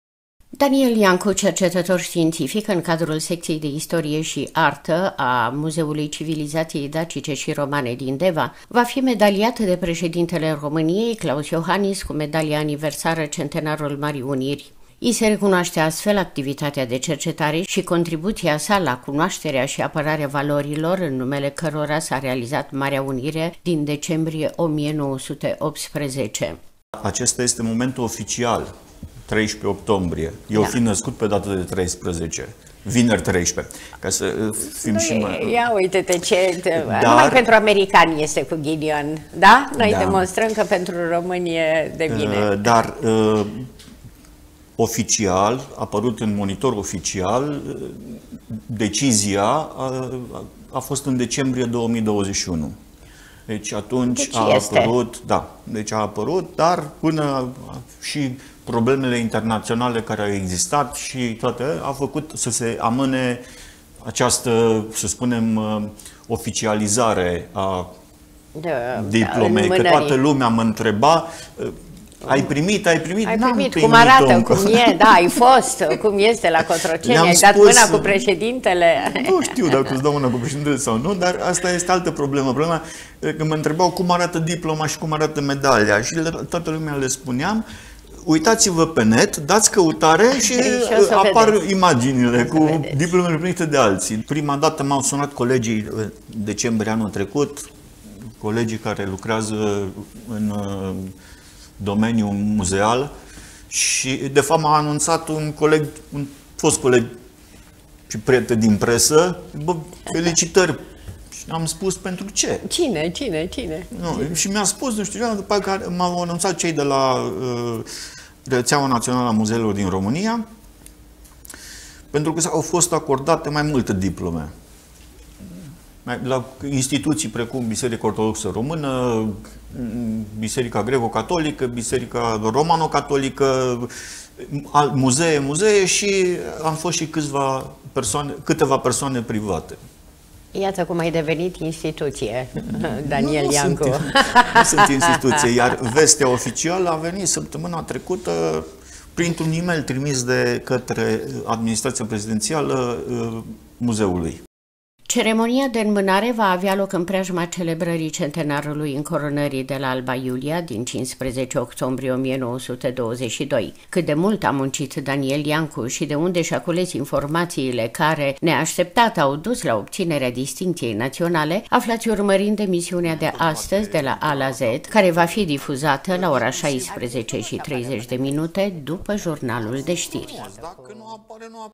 DISTINCȚIE NAȚIONALĂ PENTRU DEVEANUL DANIEL IANCU
Daniel I. Iancu, cercetător științific în cadrul Secției de Istorie și Artă a Muzeului Civilizației Dacice și Romane va fi medaliat de Președintele României, Klaus Iohannis, cu Medalia Aniversară Centenarul Marii Uniri. I se recunoaște, astfel, activitatea de cercetare și contribuția sa la cunoașterea și apărarea valorilor în numele cărora s-a realizat Marea Unire din decembrie 1918.